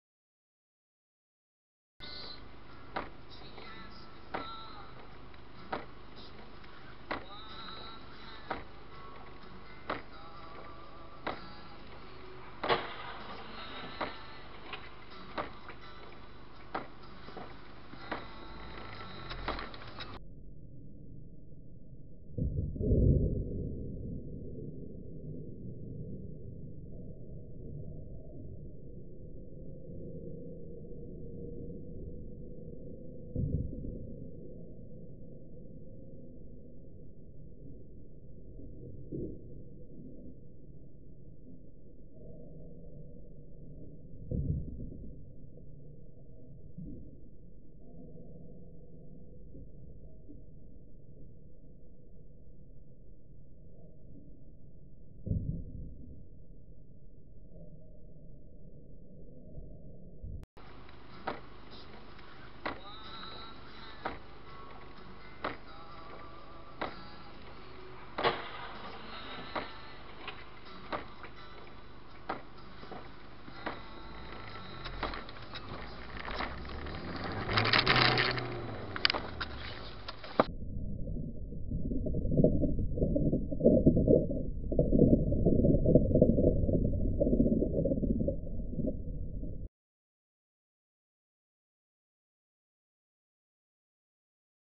Ongeval Bergen op Zoom
Ongeval dinsdagmiddag 11 februari 2014 op de Randweg Noord/Marconilaan te Bergen op Zoom.